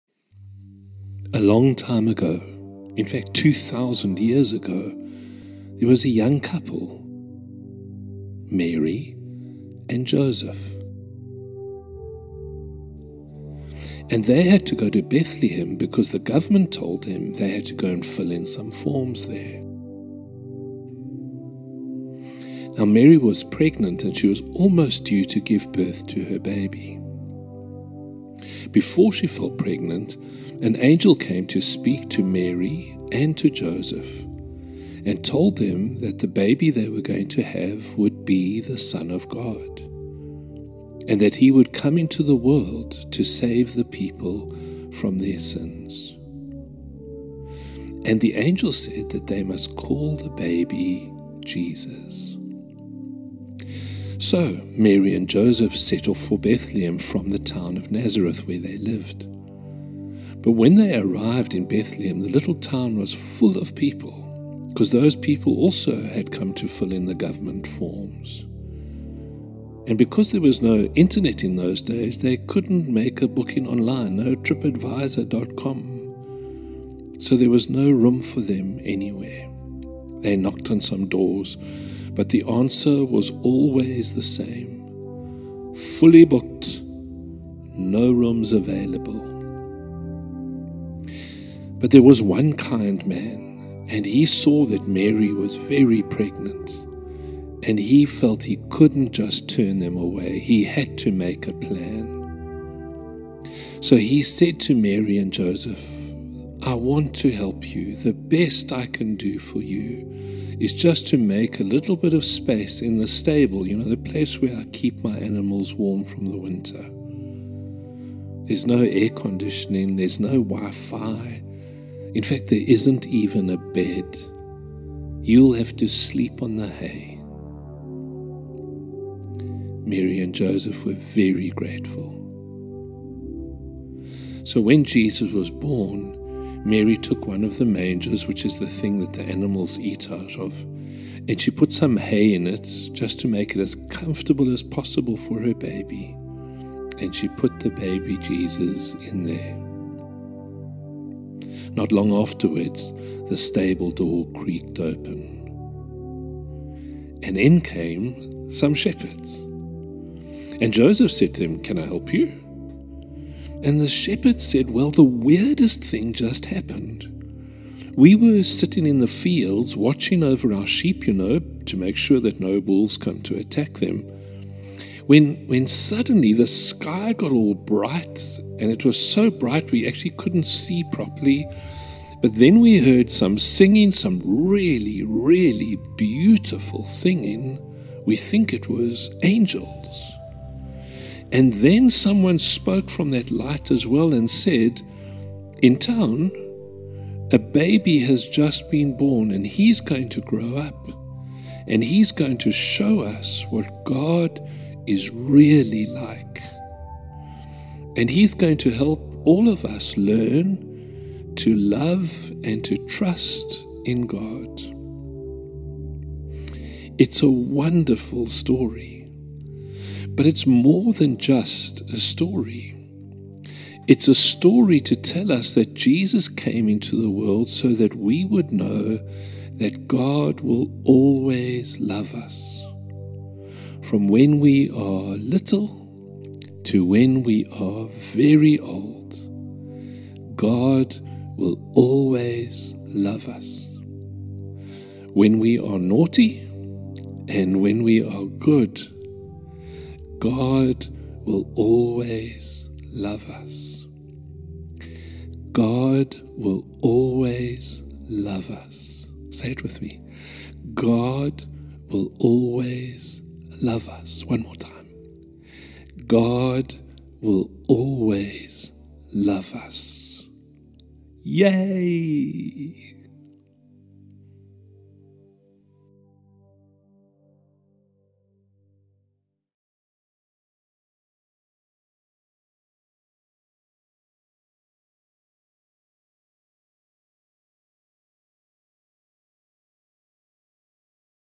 The Boy Who Loved The World ( a children's story for Christmas)
This story was used in the Christmas services of the Newton Park Methodist Church 2021